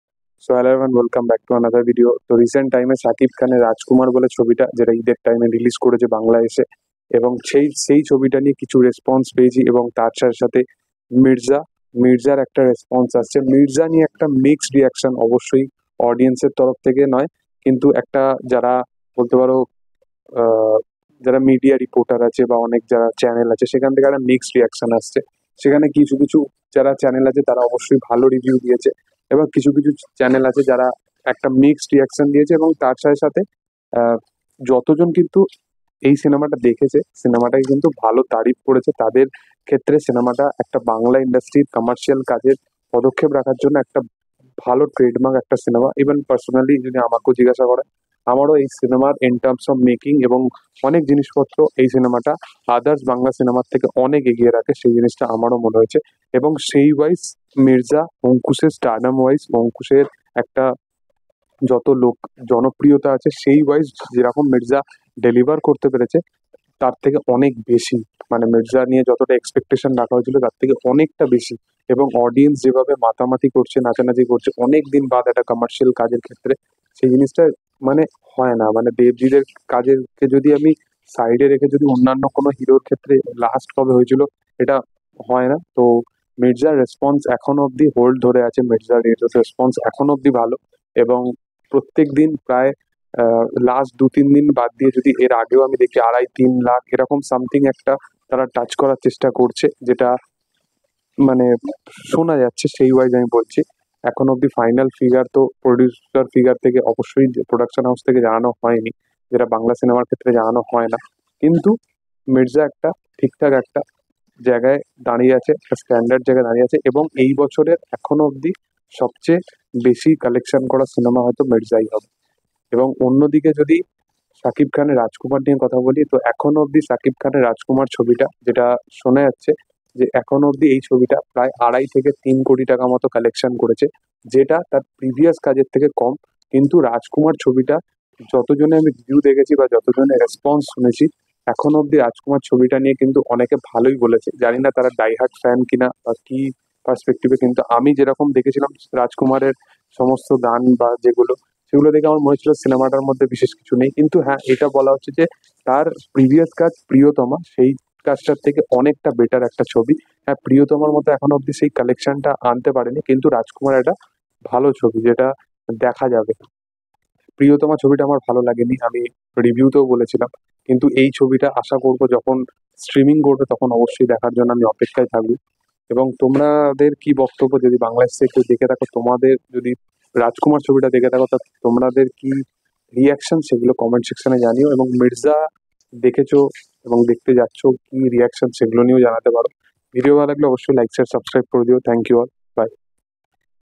অঙ্কুশ এর MIRZA FLOP? Shakib Khan Rajkumaar কি FLOP? | Ankush Hazra 🔥| Oindrila Saha
ANKUSH HAZRA | MIRZA | SHAKIB KHAN | RAJKUMAAR |